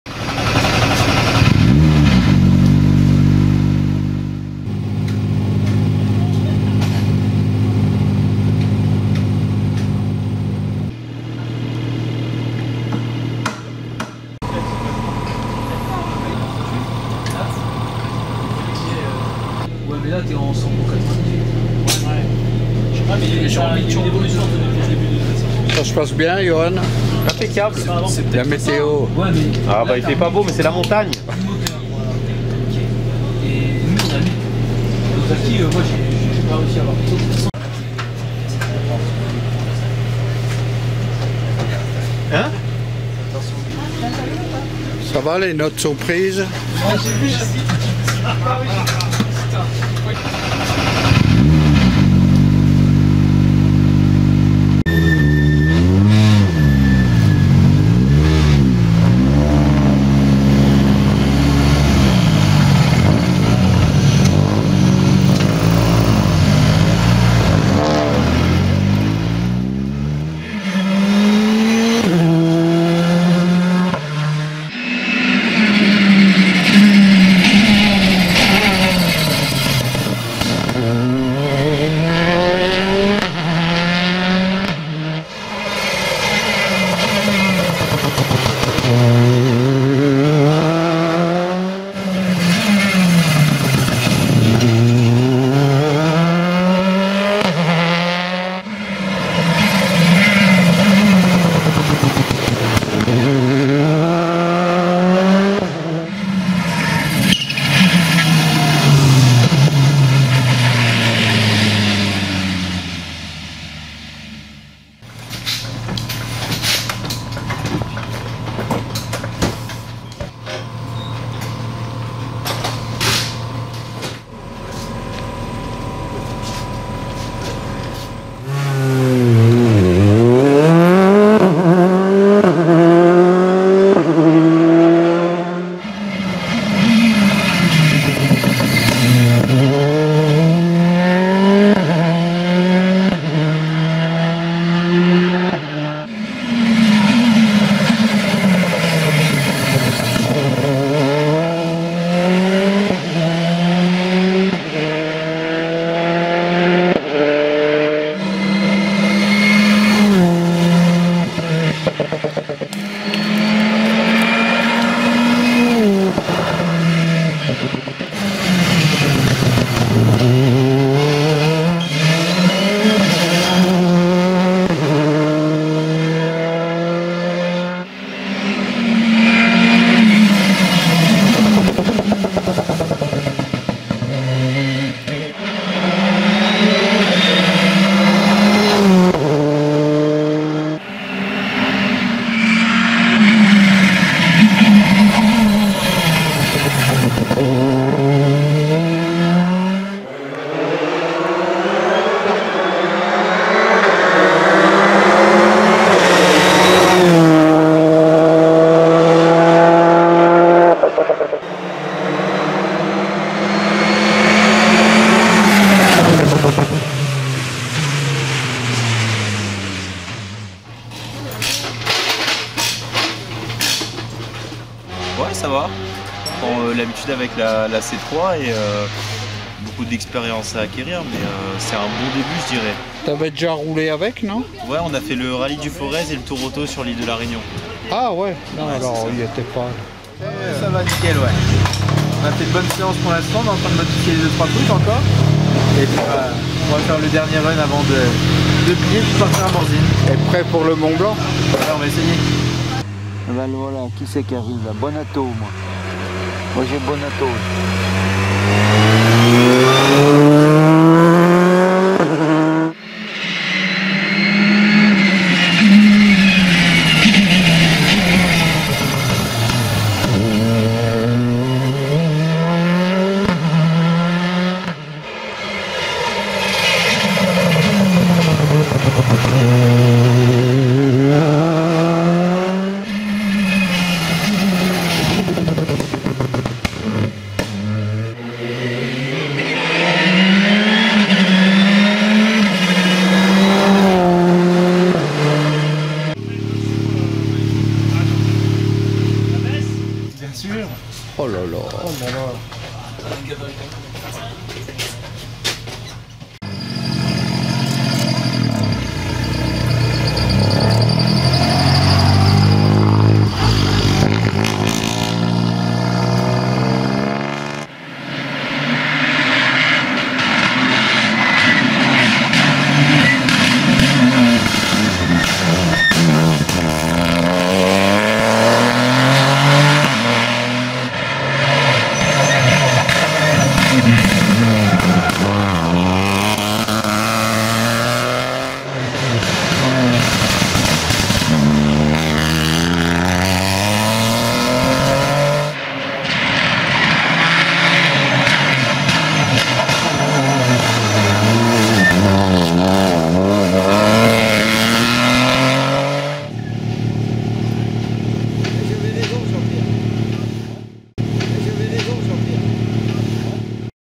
Rallye Mont blanc Morzine 2023 Tests Bonato Margaillan Gany
Tests avant rallye Mont blanc Morzine des Citroën C3 R5 de Yoann Bonato Hugo Margaillan et Rehane Gany sur une spéciale humide dans l'Ain.